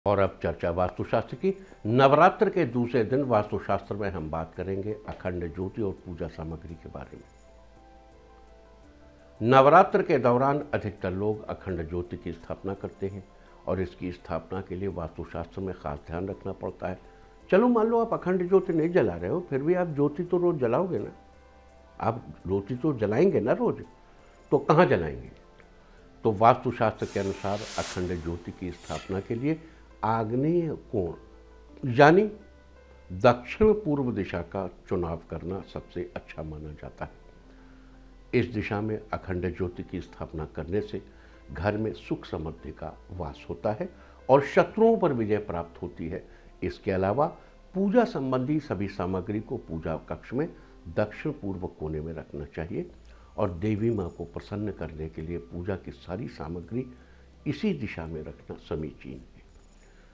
Vastu Tip of the Day: इस दिशा में भूलकर भी न जलाएं अखंड ज्योति, वरना छा जाएगी कंगाली
Vastu Tip of the Day: वास्तु शास्त्र के अनुसार अखंड ज्योति की स्थापना के लिये आग्नेय कोण, यानि दक्षिण-पूर्व दिशा का चुनाव करना सबसे अच्छा माना जाता है।